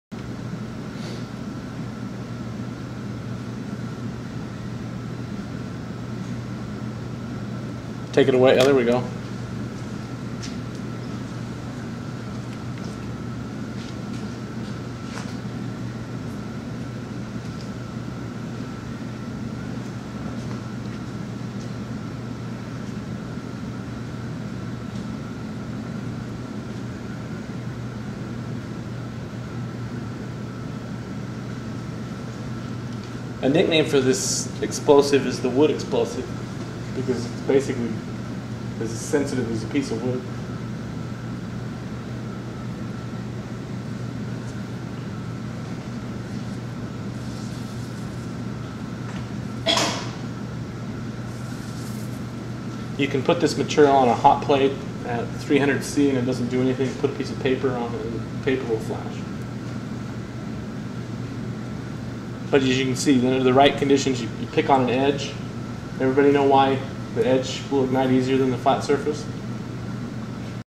PBX 9502 (TATB based plastic bonded explosive)
Notice how hard it is to ignite this material.